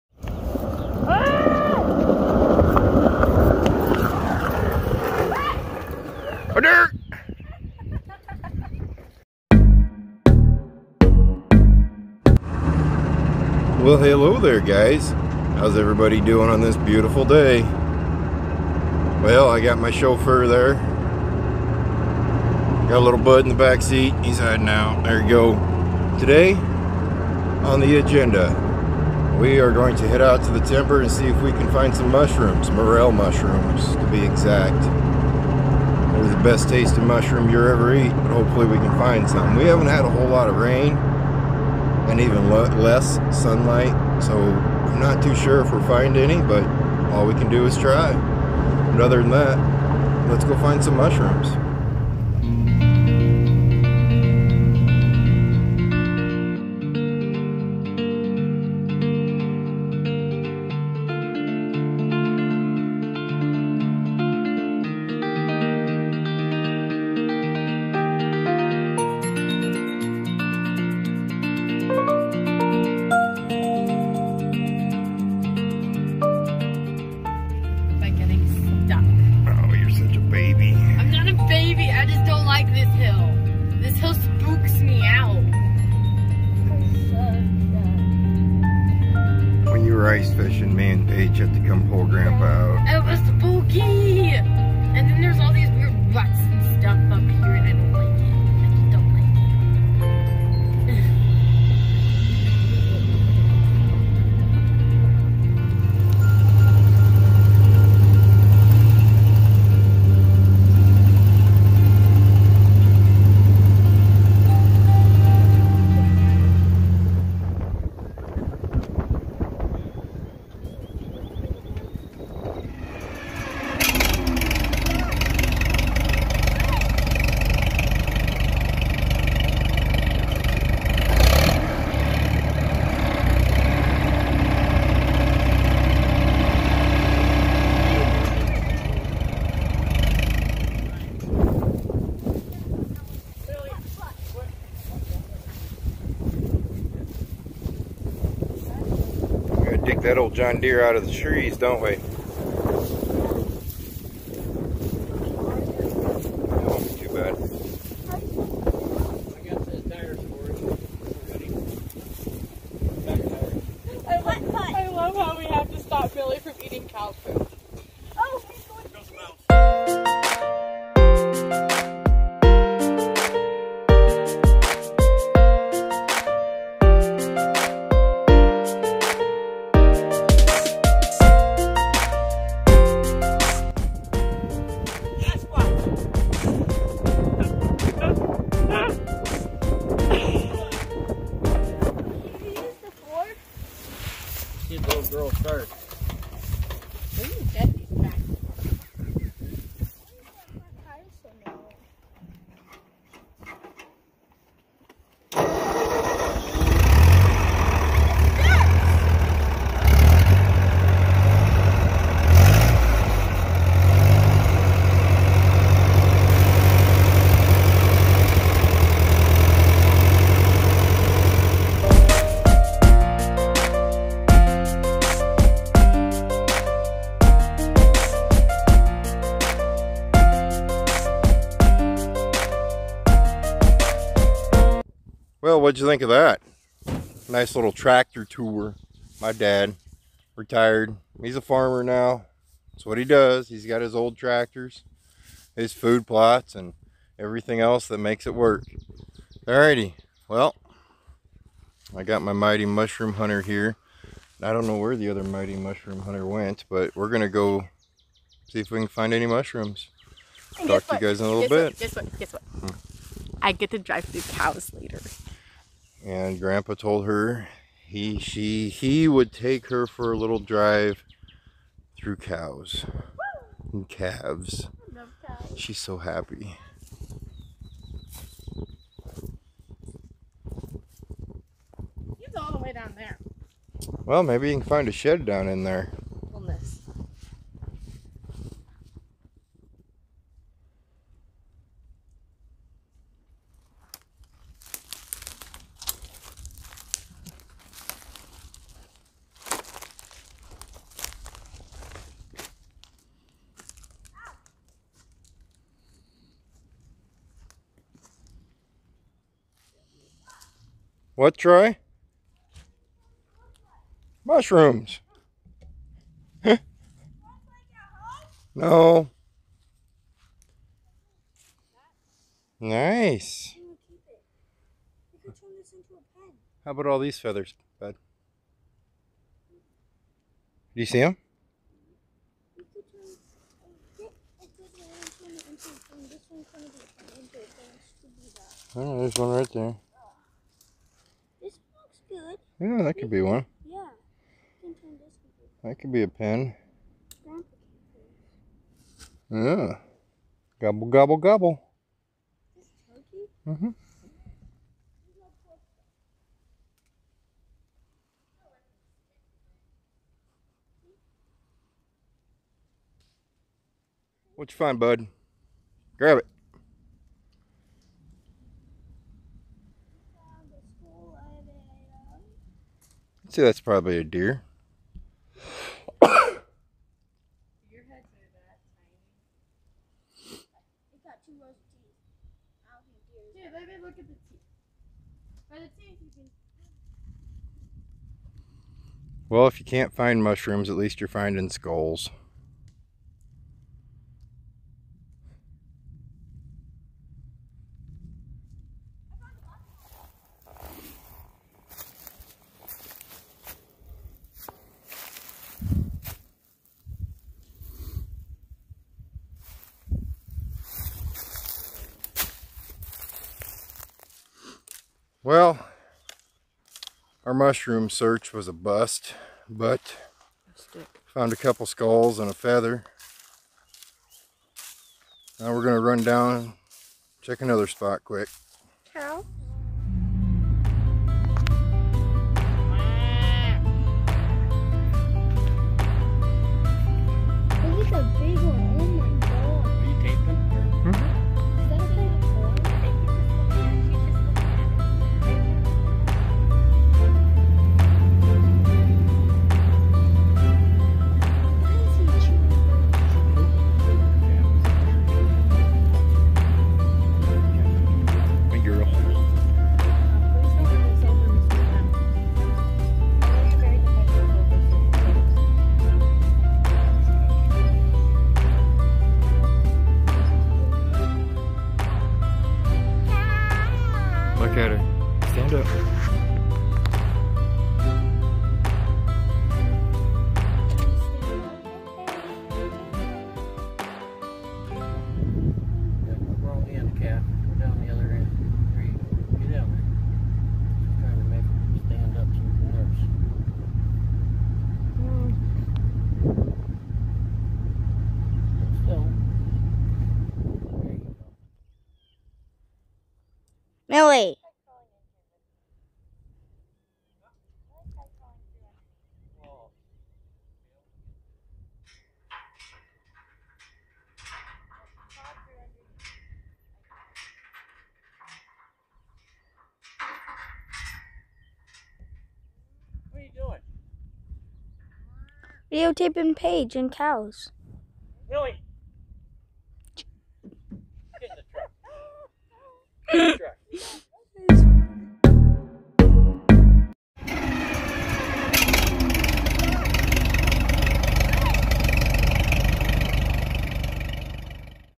Our Own Tractor Museum!!!!
We go on a great hunt for mushrooms. Before we go hunting we take a look at grandpas treasures! Its our own tractor museum. Then we go for a stroll through some cows and get to see a calf be born!!! Enjoy